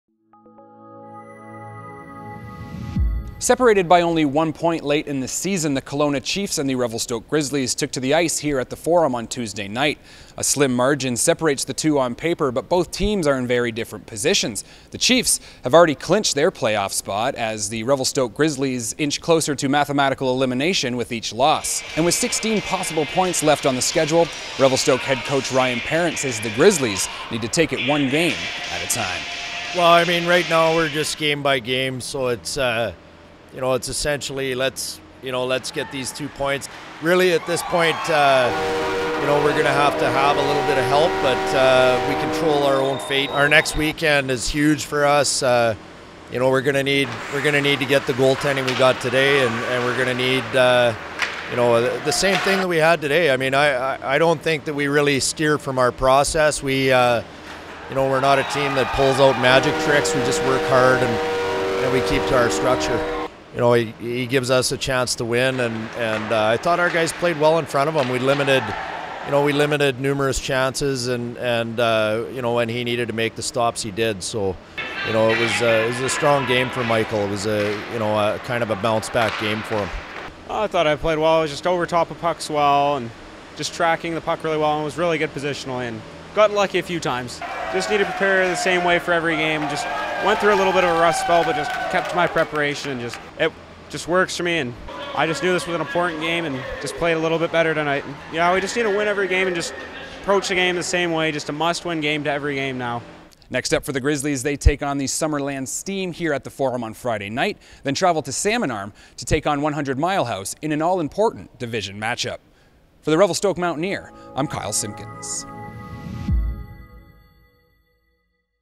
Revelstoke Grizzlies down Kelowna Chiefs 5-0 on Jan. 27
Kyle Simkins reports for the Revelstoke Mountaineer on the Revelstoke Grizzlies convincing win over the Kelowna Chiefs on Jan. 27.